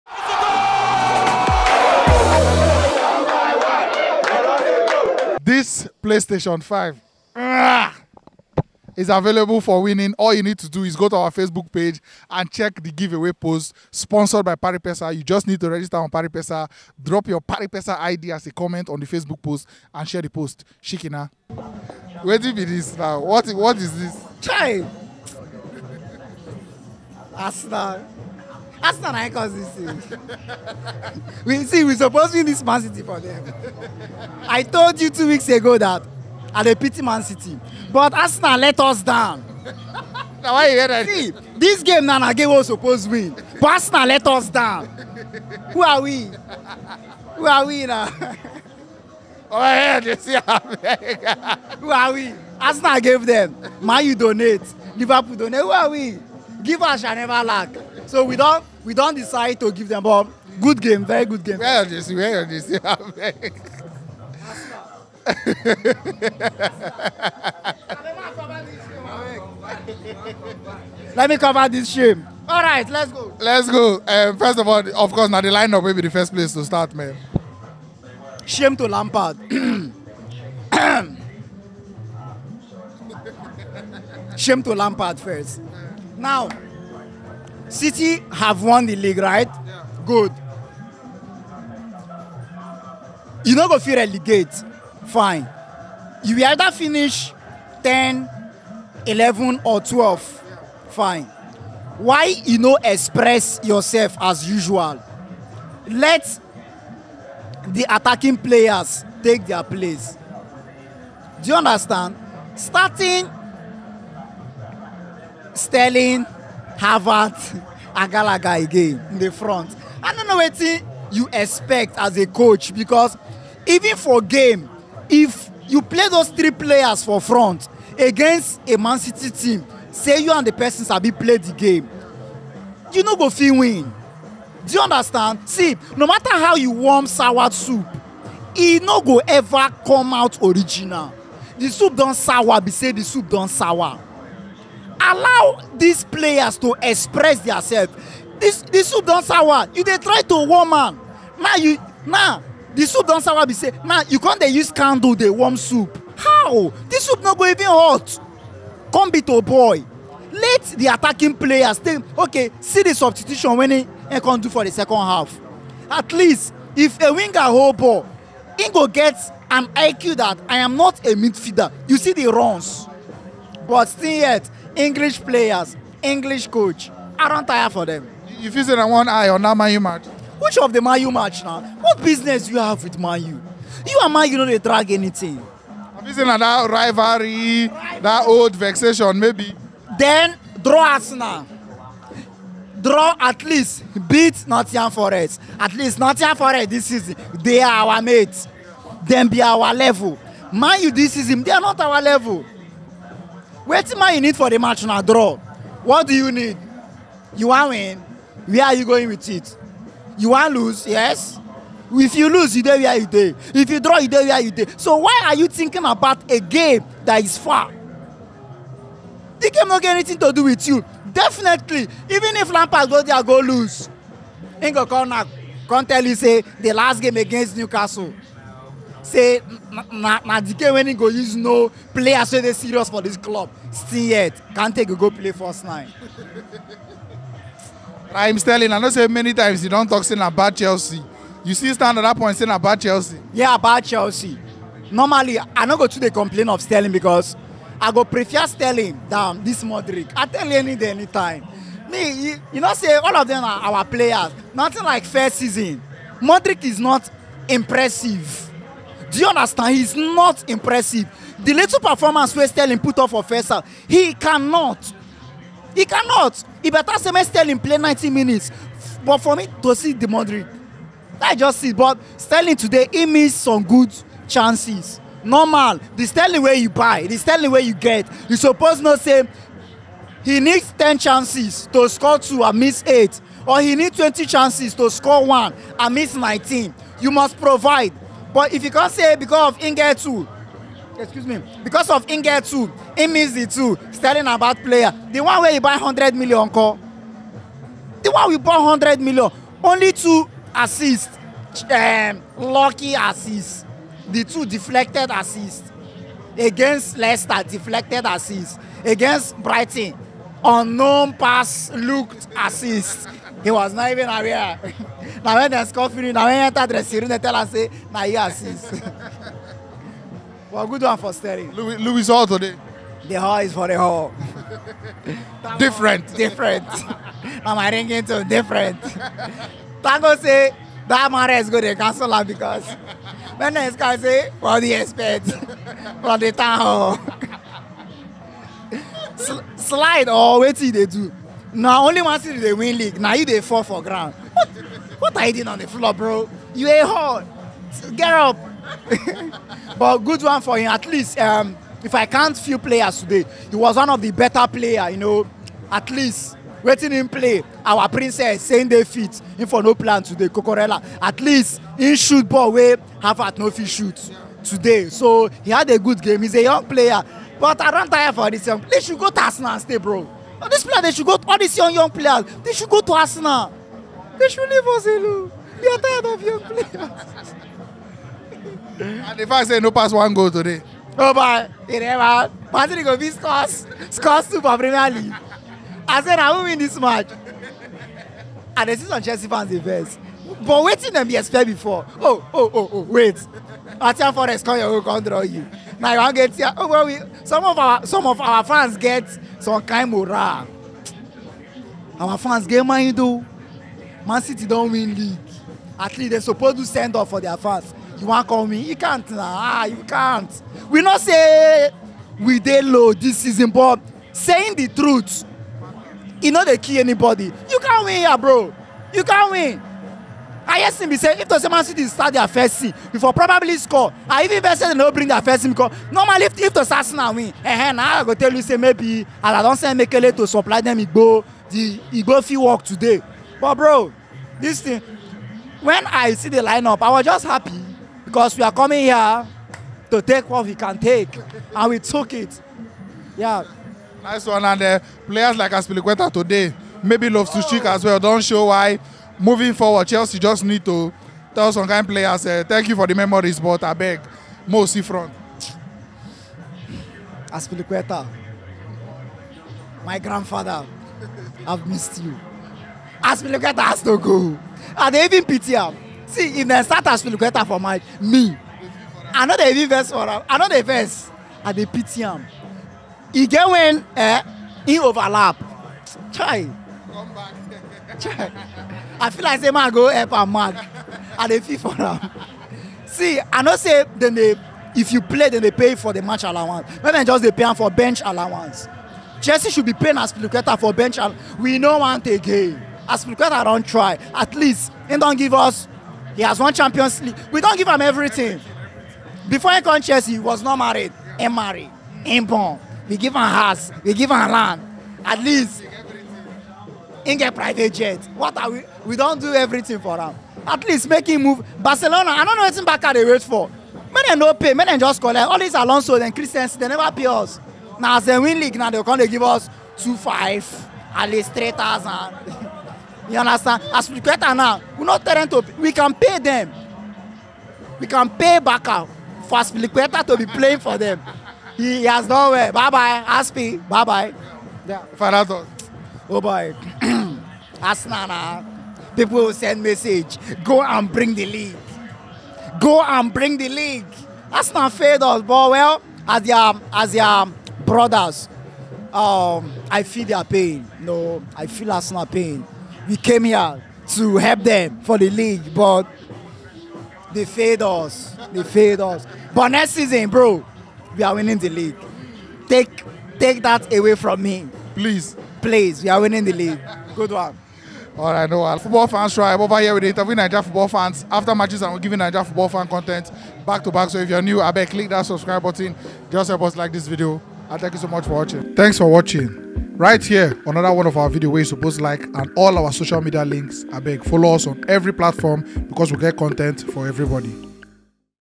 MANCHESTER CITY 1-0 CHELSEA ( Dani - NIGERIAN FAN REACTION) - PREMIER LEAGUE HIGHLIGHTS 2022-23
✅ The Winner will also be invited on June 10th to our location to watch the Champions League final with us and pickup the prize!

Match Reaction from Dani, as e say Chelsea wan been do am for Arsenal, Lampard playing Gallagher, Sterling and Havertz for attack na mumu ting, e suppose free d players, etc.

Proposed next Game 👇🏽
EPL